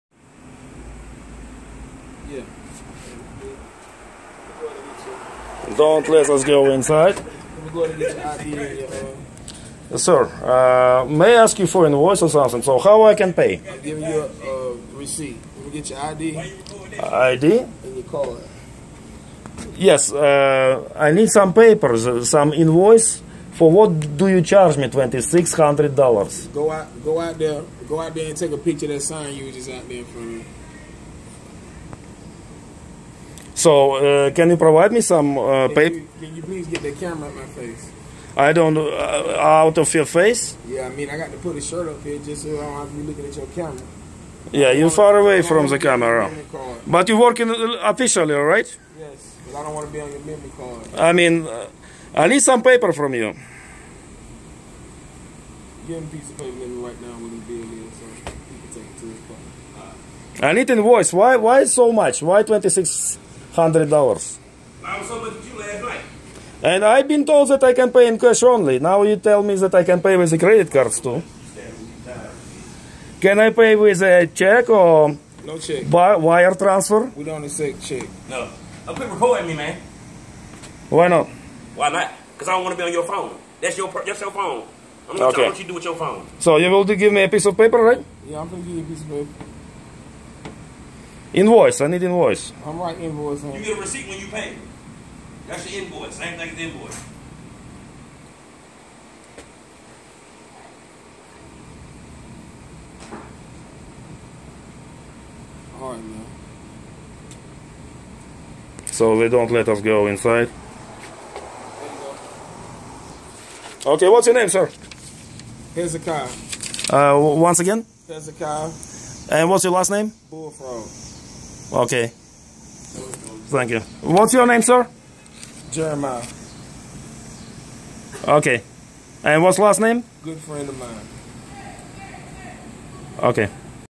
Carolina's towing LLC office. They wanted me to put $2,600 in cash on the bottom of the window
Carolina's towing LLC office.  They told me to put $2,600 in cash on the bottom of the window to take back my truck